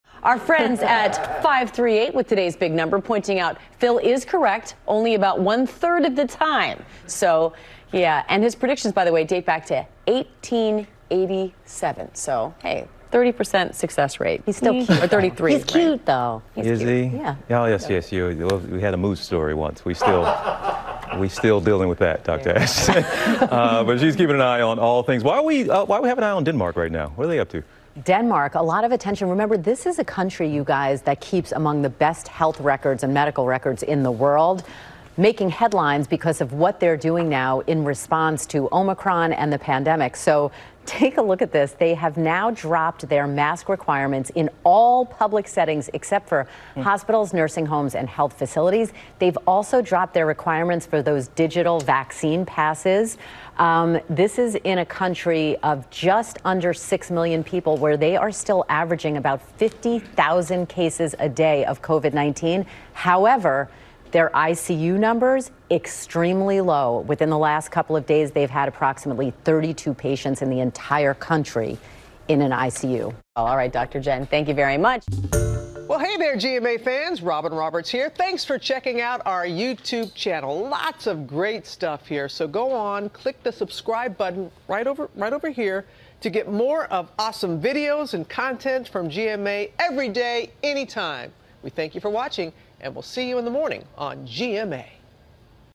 Pandemic response in Denmark
As COVID-19 cases there average 50,000 a day, Denmark officials say they’re dropping mask mandates everywhere except hospitals, nursing homes and health facilities.